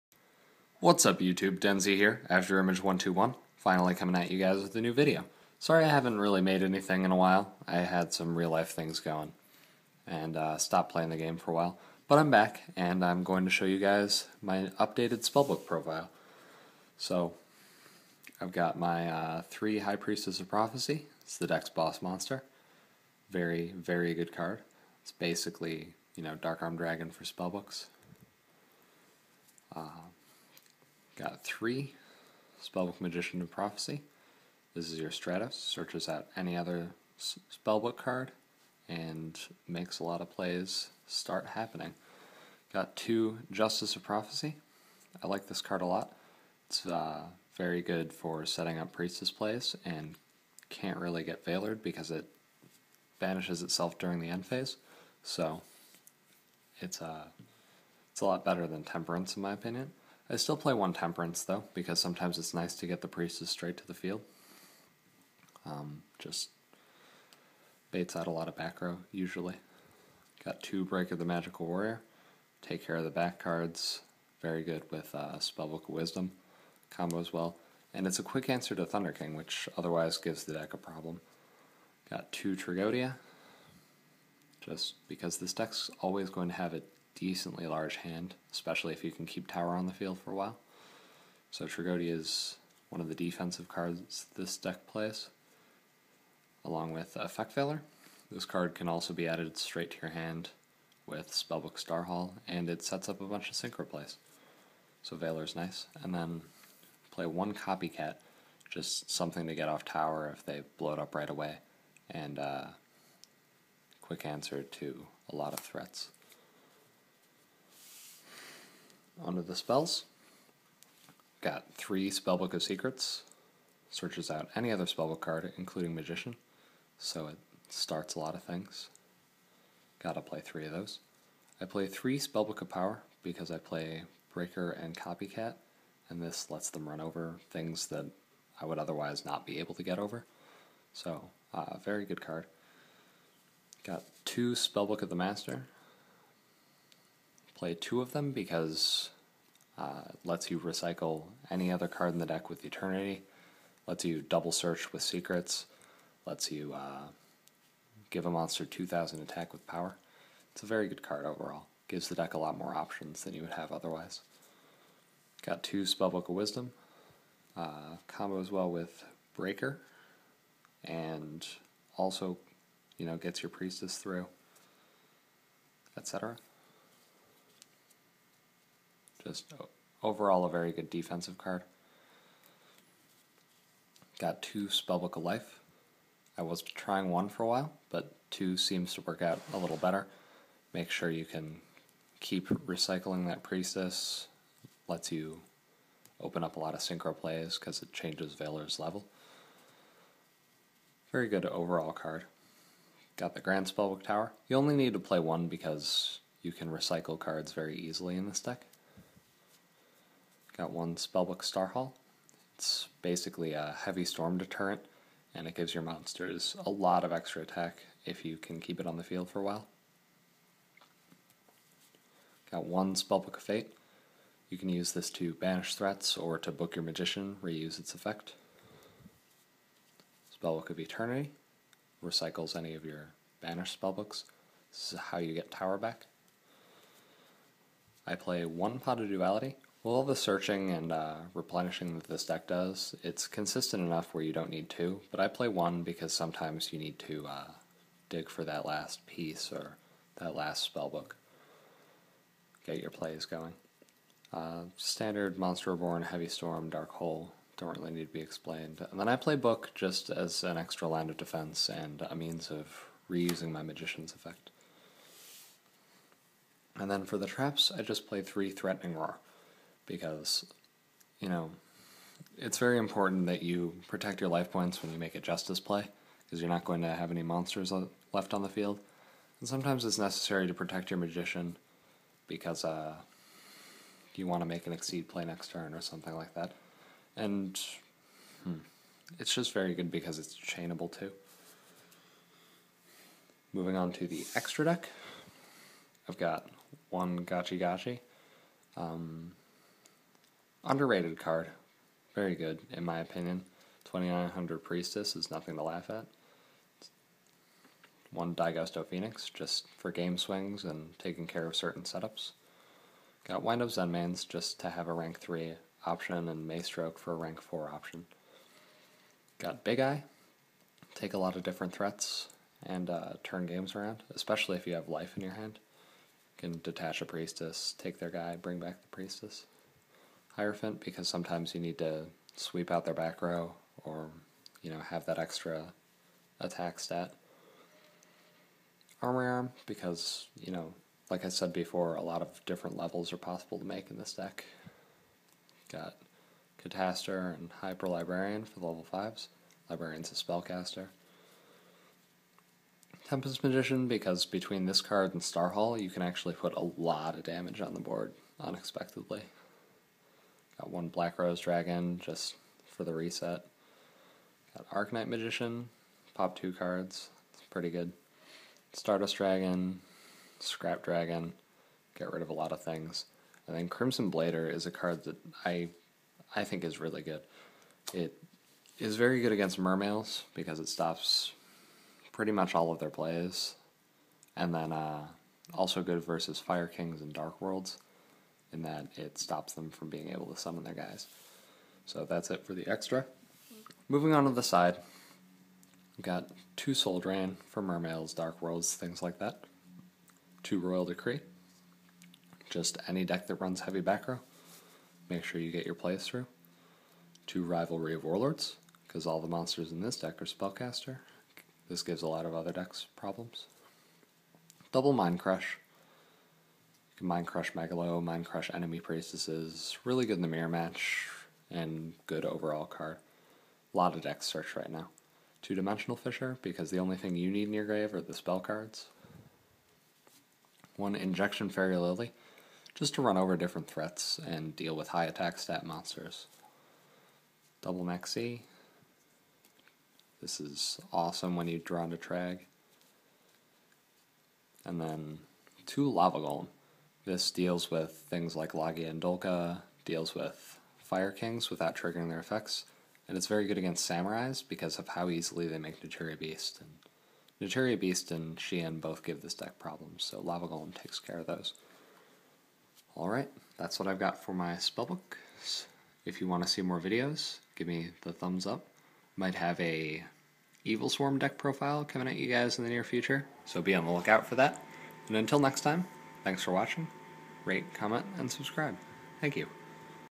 Updated Spellbook Deck Profile!
Hey guys, sorry about the long break. I'm diving rigt back in to making videos, starting off with my updated Spellbook deck! Let me know what you guys think.